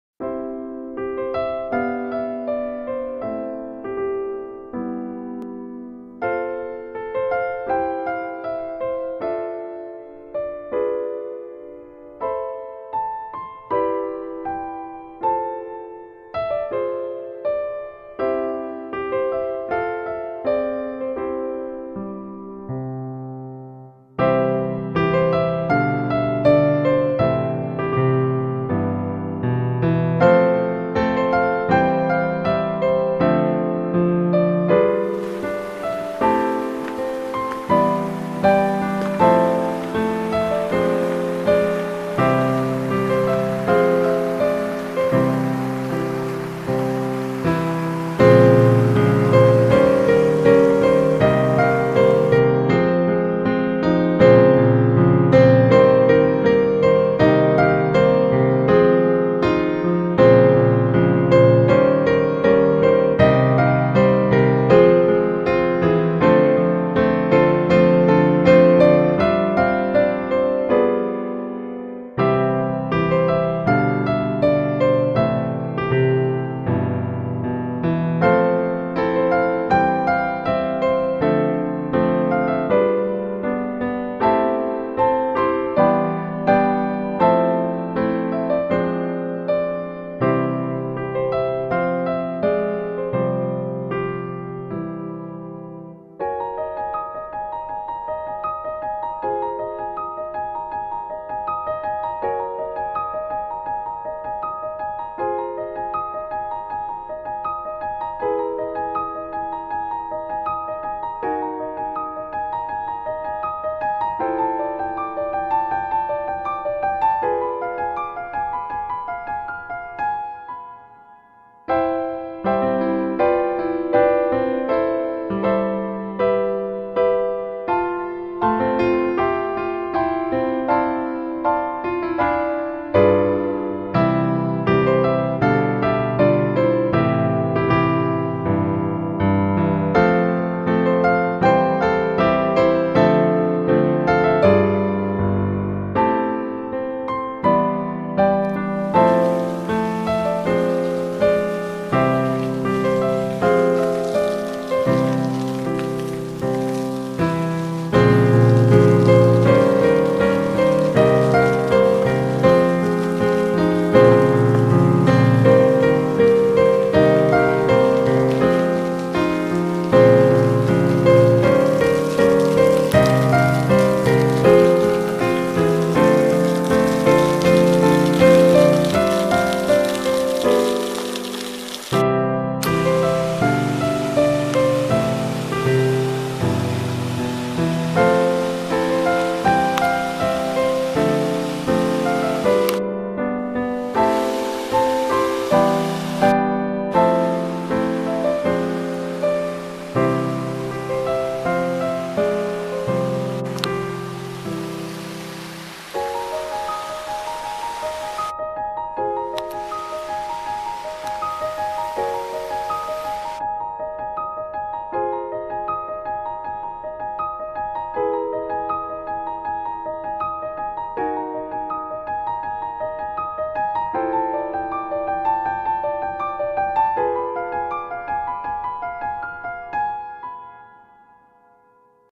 2022 1少しだけ雪が降った兼六園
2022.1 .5の兼六園。早朝は誰でも無料入園です。雪は降ったりやんだり。BGMはMUSMUS　金沢　兼六園